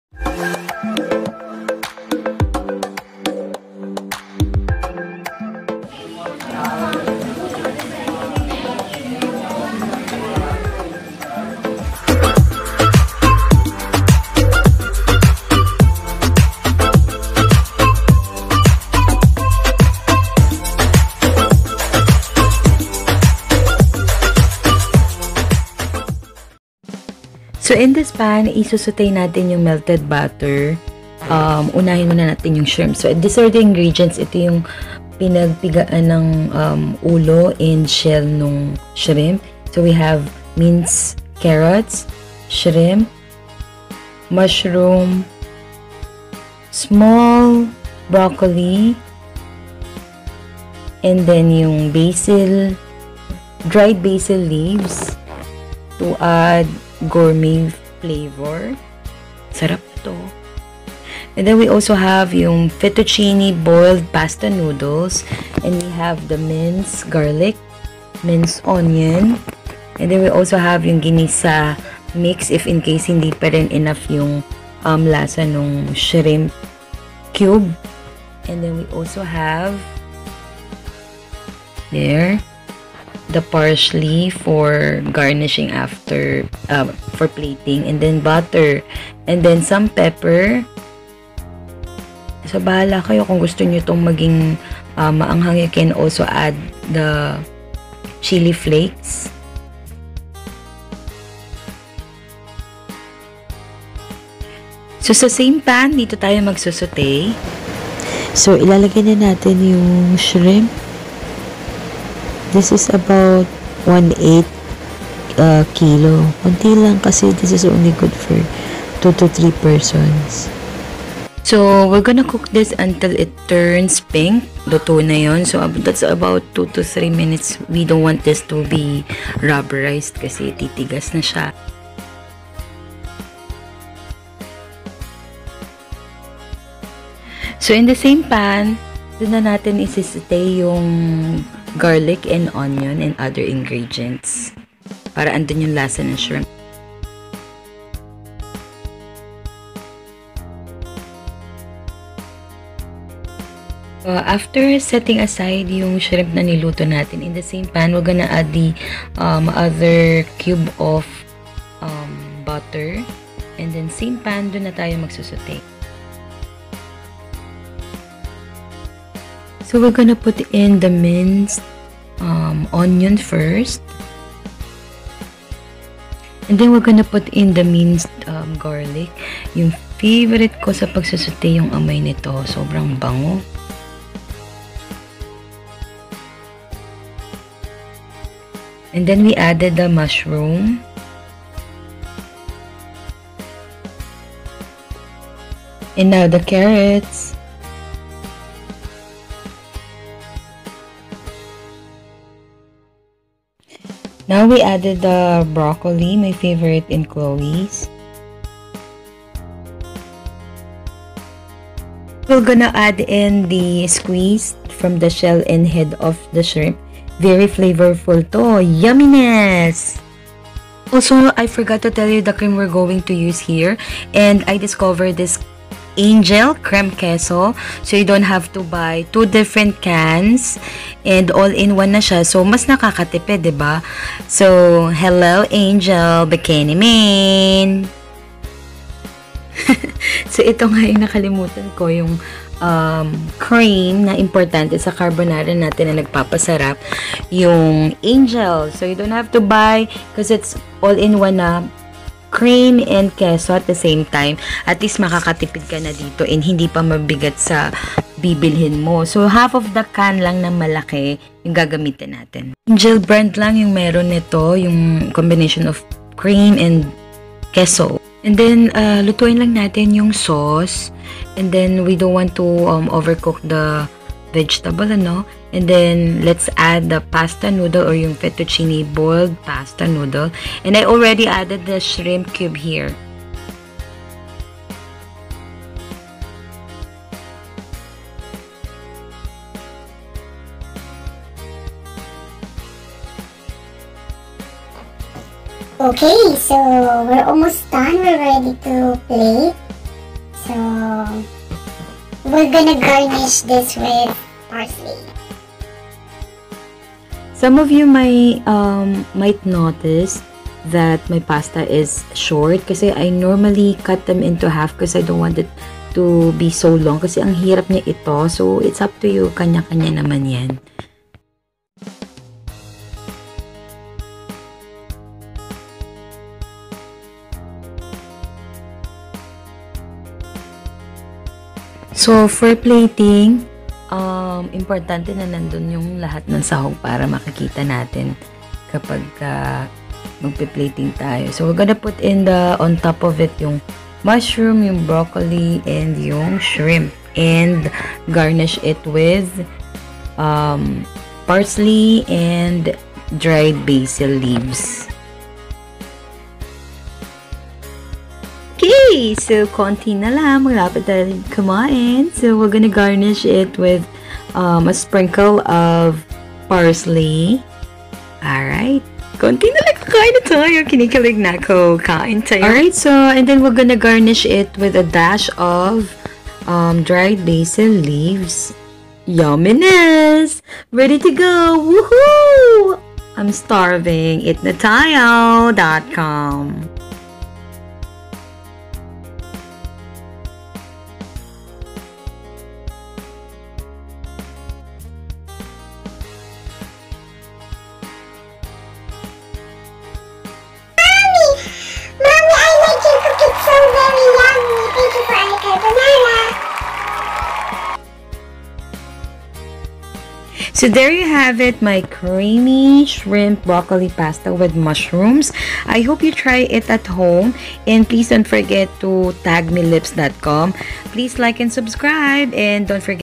Creamy Shrimp Broccoli Pasta with Mushrooms | Creamy Carbonara
Sharing you one of the recipes I love to eat and cook most of the time. My Creamy Shrimp Brocolli Pasta with Mushrooms. U can also use breast chicken if you don't want shrimps.

Tag me if u try this using lipsdotcom and comment your thoughts and how it went when u tried this with your loved ones.

Hope you enjoyed the video and give it  a 👍.